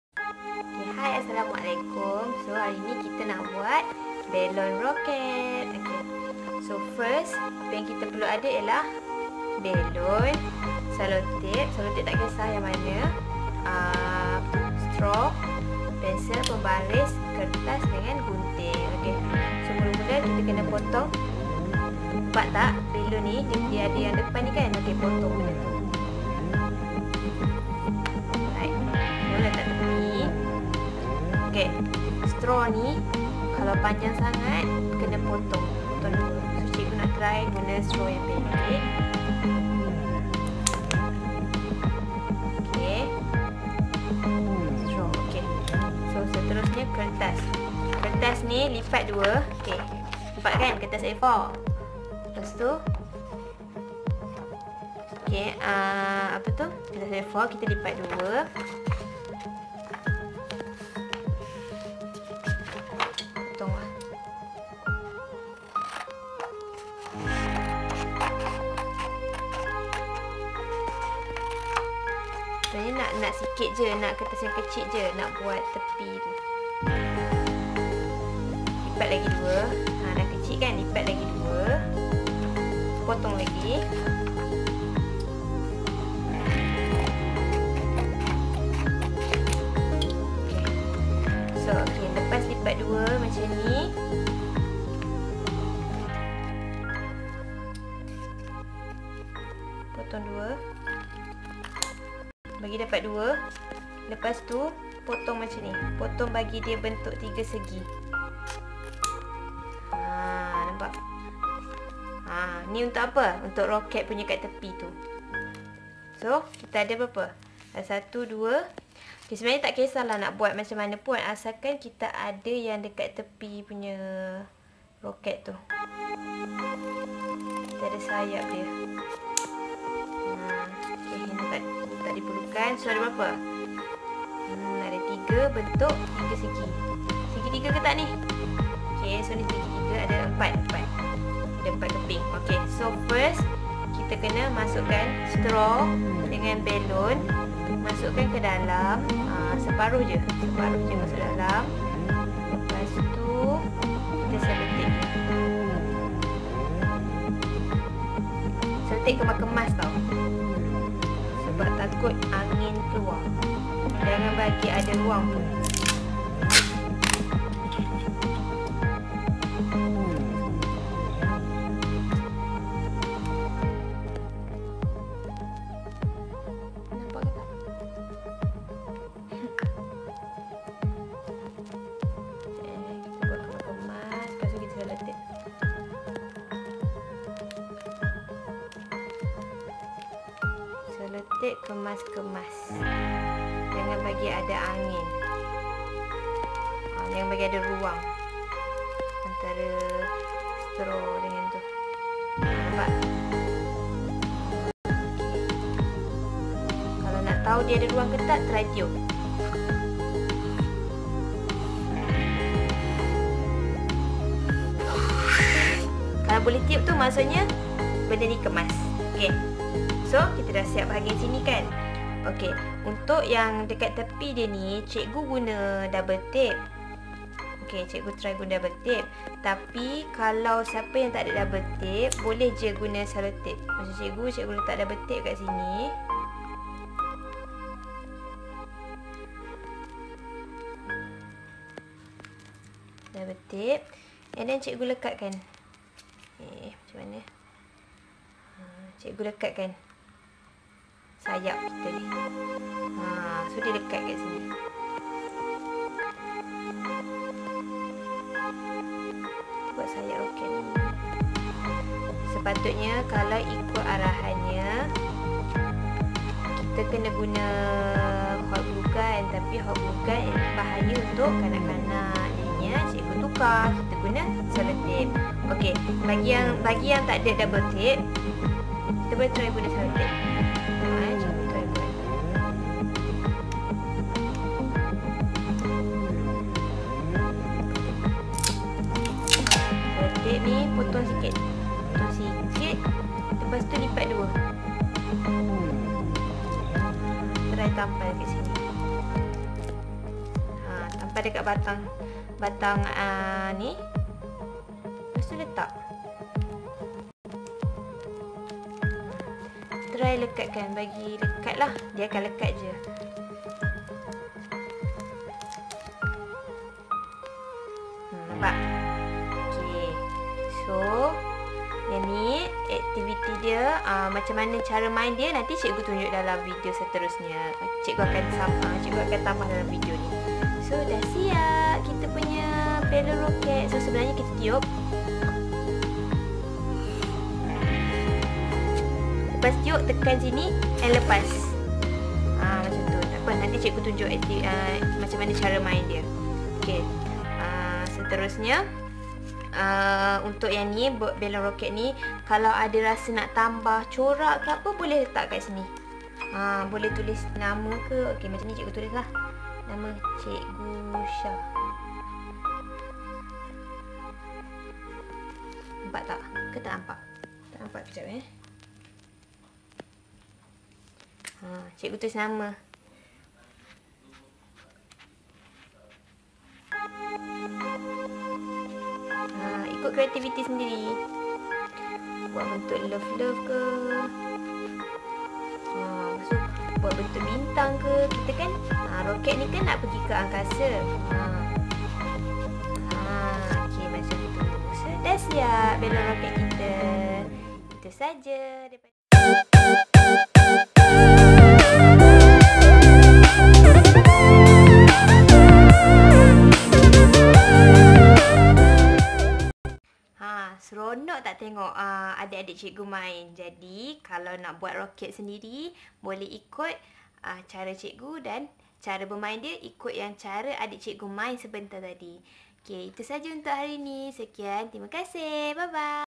Cara Buat Belon Roket DIDI & FRIENDS 🎈🚀 (How to make Balloon Rocket DIDI & FRIENDS) 🥳
Cara buat roket balloon Didi & Friends ♥️
Aktiviti ini sesuai untuk Kanak-Kanak berumur 4 tahun hingga 12 tahun ✨

Khas buat anak murid Cikgu Sya di PraPintar SK Bangsar ♥️